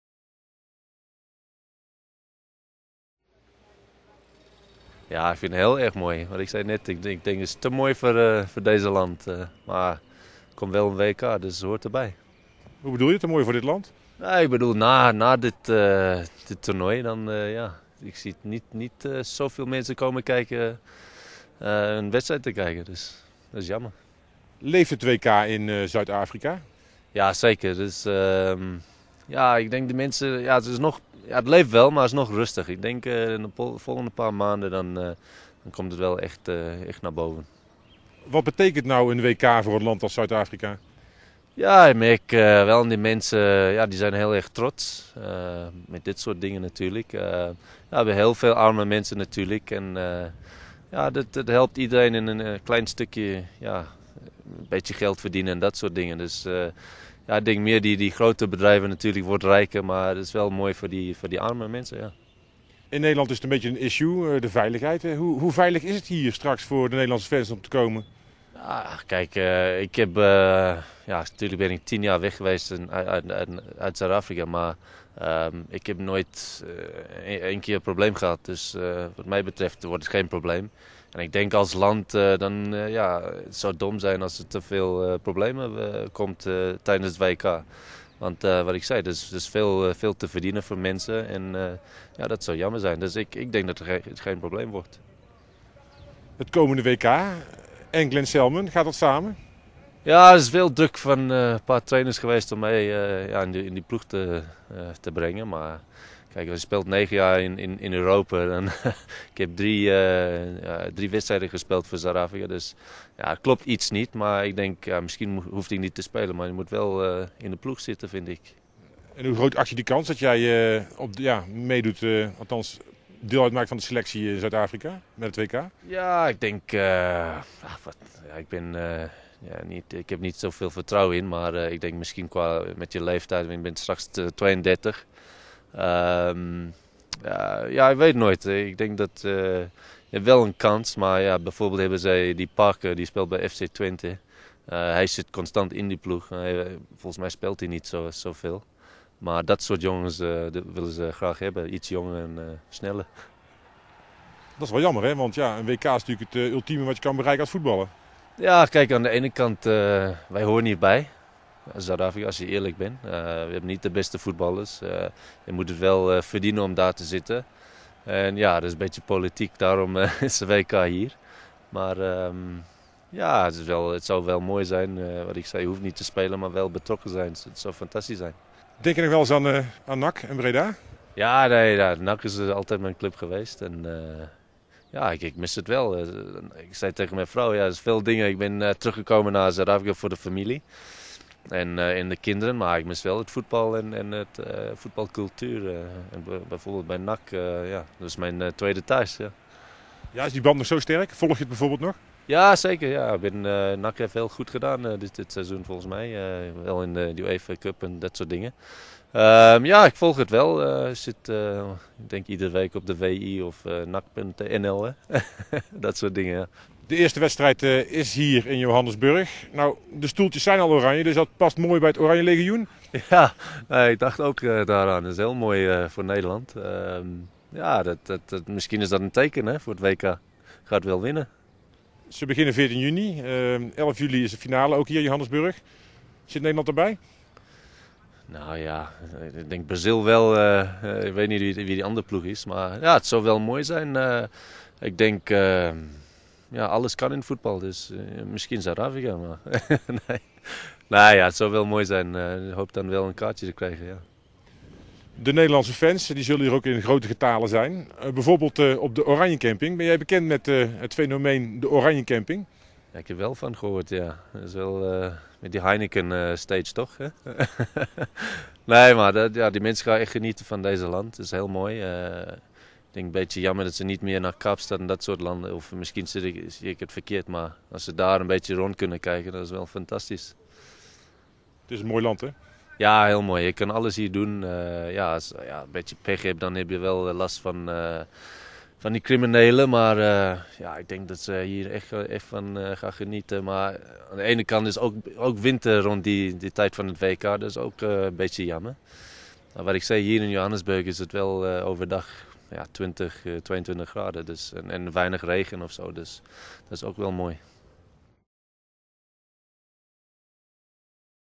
Glen Salmon over het WK en Supersport United
Op de fans van NAC heeft Glen Salmon een onvergetelijke indruk gemaakt. De Spits uit Zuid-Afrika speelde tussen 1998 en 2007 vijfenhalf seizoen voor de Bredase ploe en scoorde 36 keer. Op dit moment is Salmon actief voor de club Supersport United in Pretoria. Hij hoopt komende zomer met de Zuid-Afrikaanse ploeg het WK te spelen.